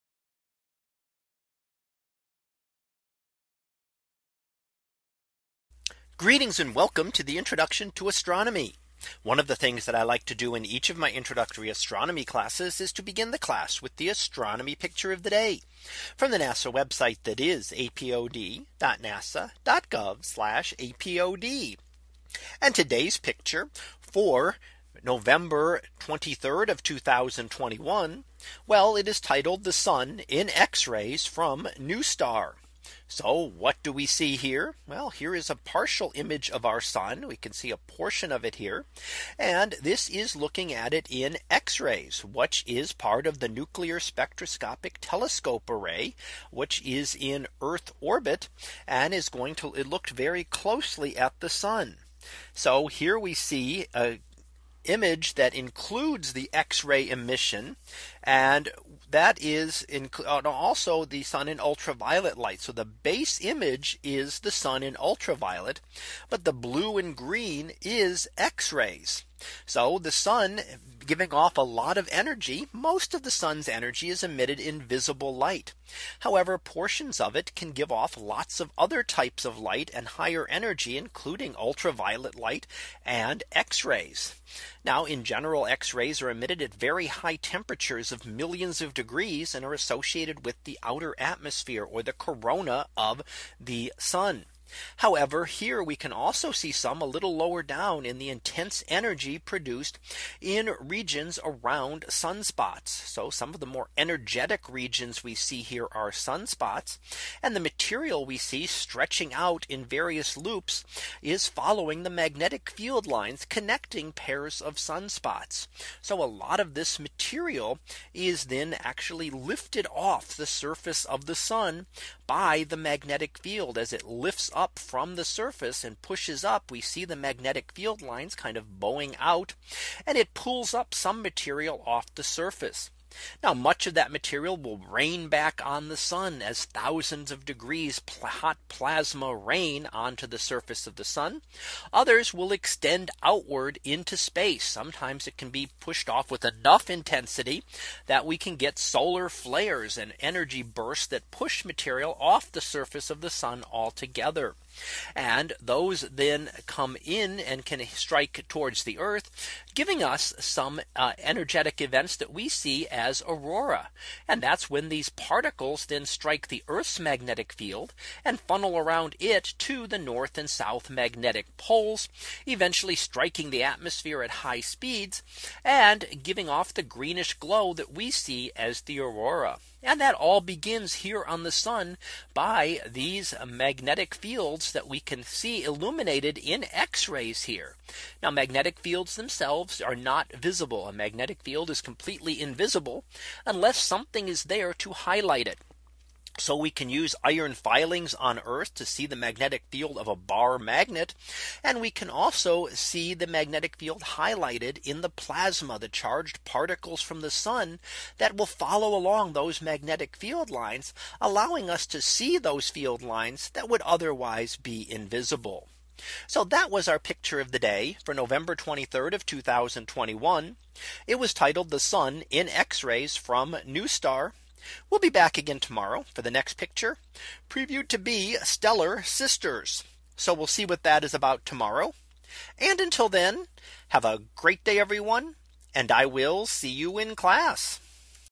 2021 November 23 - The Sun X-rays from NuSTAR
In today's image, we see an image of the Sun in ultraviolet light with enhancements showing the x-ray emission taken by the NuSTAR telescope.